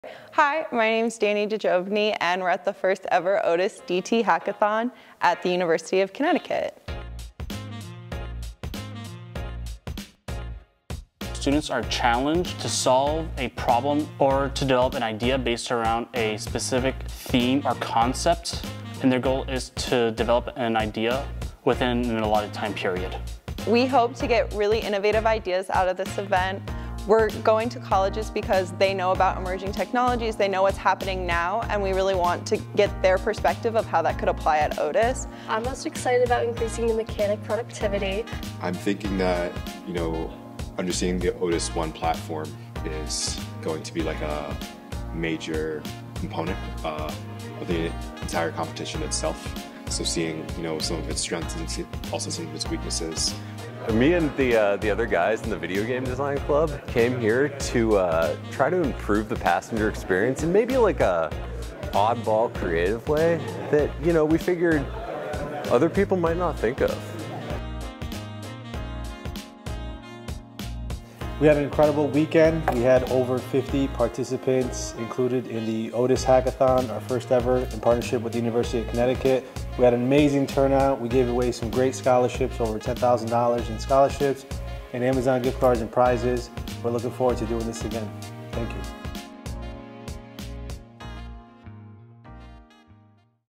Otis Innovation Hackathon at the University of Connecticut
Congratulations to all of the University of Connecticut students who participated in the Otis Innovation Hackathon this weekend! It was exciting to see these young minds at work using STEM-based problem solving and leadership skills. Thank you to everyone who made this event a success.